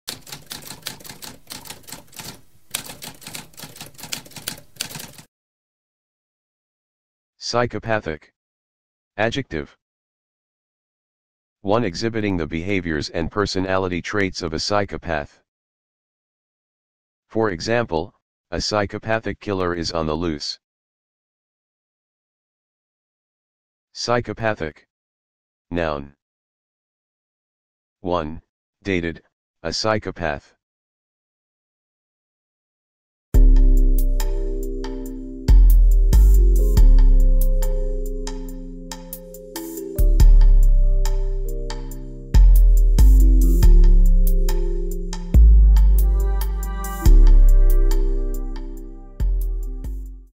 What does psychopathic mean?
A spoken definition of psychopathic.

Intro Sound:
Typewriter - Tamskp